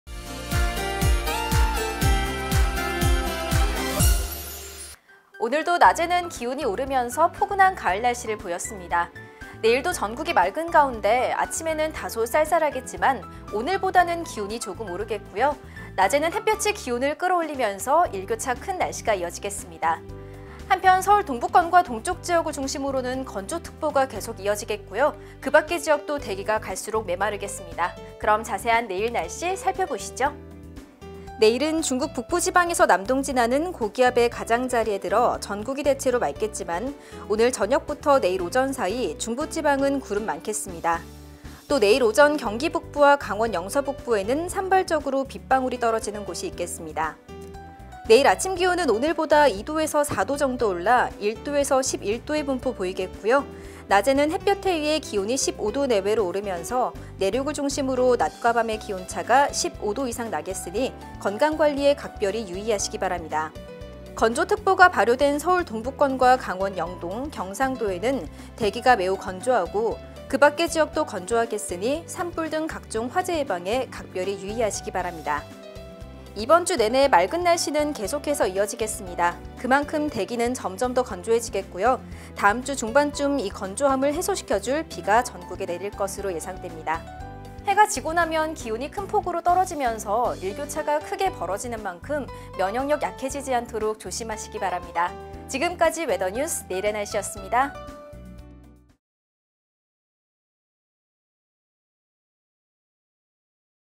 [웨더뉴스] 내일의 날씨 (11월 13일)
오늘도 낮에는 기온이 오르면서 포근한 가을 날씨를 보였습니다.

내일도 전국이 맑은 가운데 아침에는 다소 쌀쌀하겠지만 오늘보다는 기온이 조금 오르겠고요. 낮에는 햇볕이 기온을 끌어올리면서 일교차 큰 날씨가 이어지겠습니다.

한편, 서울 동북권과 동쪽 지역을 중심으로는 건조특보가 계속 이어지겠고요. 그 밖의 지역도 대기가 갈수록 메마르겠습니다.

오늘의 캐스터 : 이현서